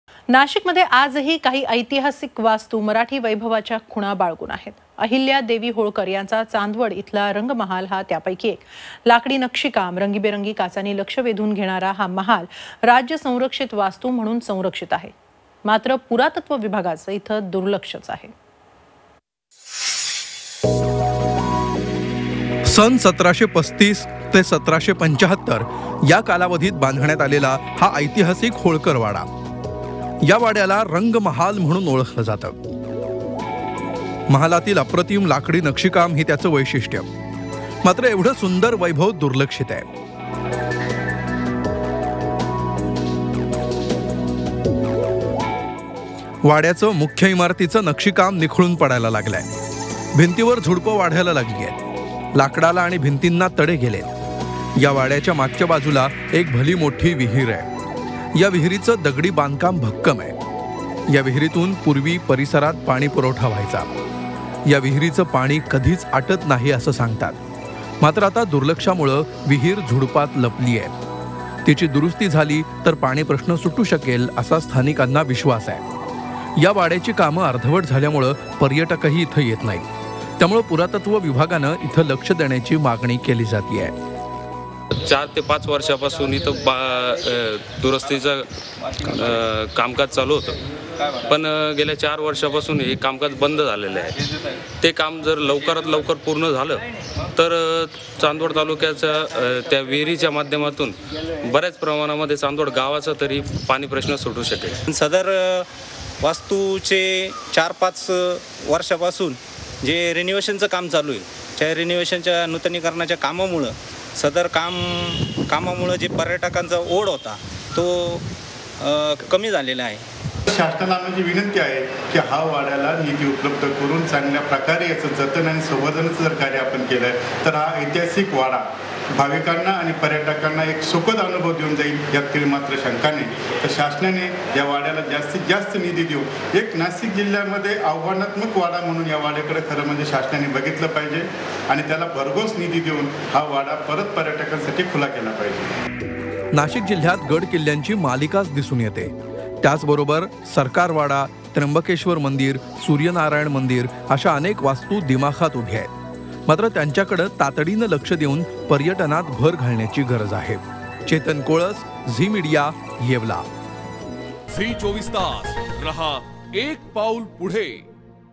नाशिक | आहिल्यादेवी होळकरांची वास्तू झाली जीर्ण
Nashik | Chandwad | Majestic And Artistic Rang Mahal In Poor Condition